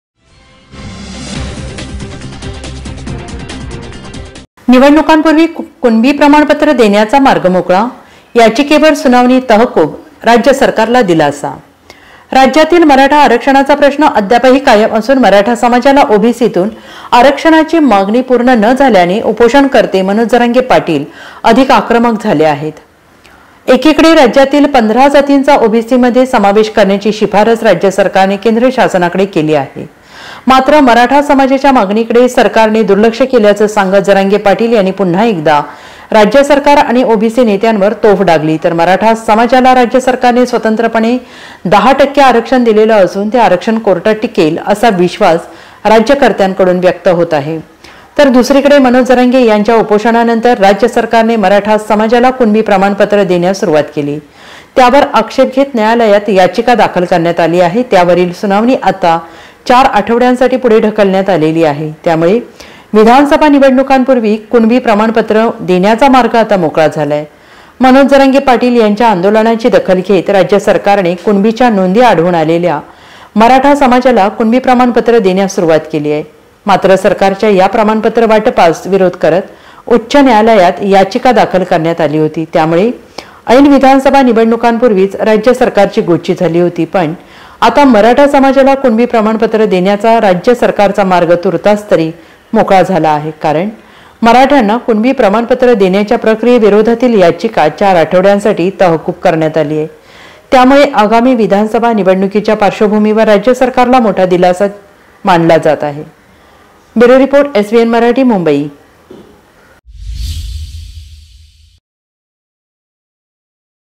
MUMBAI निवडणुकांपूर्वी कुणबी प्रमाणपत्र देण्याचा मार्ग मोकळा; याचिकेवर सुनावणी तहकूब
निवडणुकांपूर्वी कुणबी प्रमाणपत्र देण्याचा मार्ग मोकळा; याचिकेवर सुनावणी तहकूब, राज्य सरकारला दिलासा.